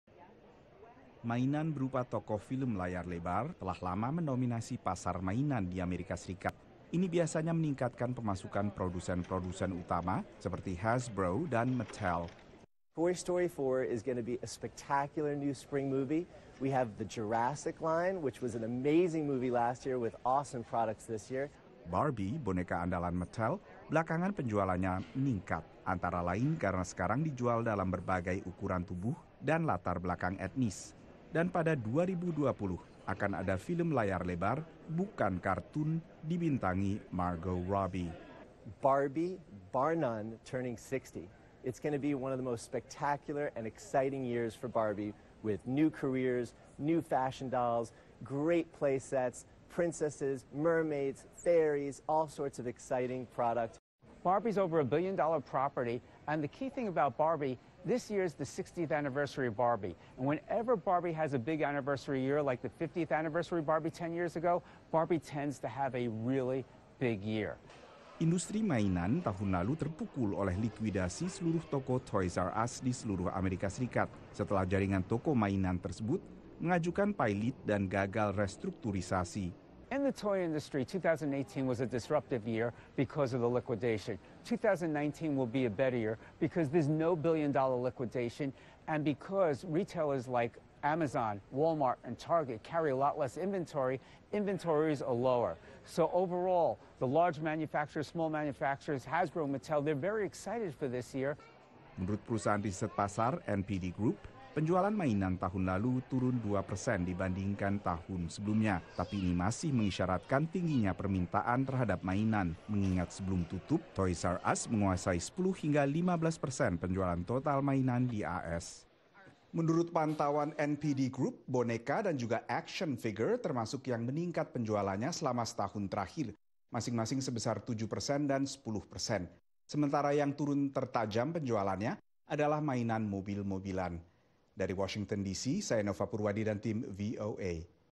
Produsen Mainan Siap Bangkit Kembali Setelah Pailitnya 'Toys"R"Us'
Bangkrutnya jaringan toko mainan 'Toys"R"Us' di AS dan Kanada ternyata menekan penjualan mainan di Amerika tahun lalu. Tapi tingginya permintaan akan mainan terutama menjelang Natal dan tahun baru atau peluncuran film baru, penjualan tahun ini diperkirakan akan kembali menguat.